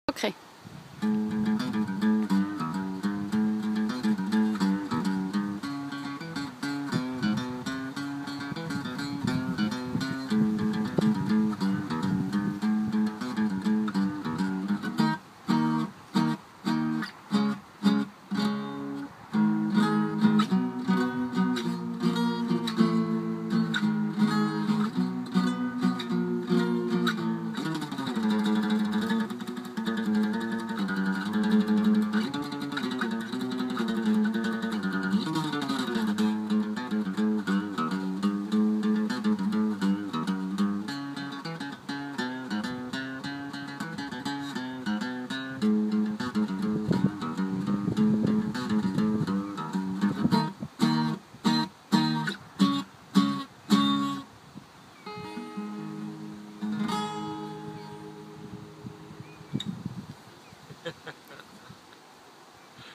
Nordic Campers Matthias Hammerly Solo
Matthias Hämmerly playing the guitar on his trip around Norway on a car
We are so lucky to get to meet people like Matthias and Fabienne in our Nordic Campers adventure.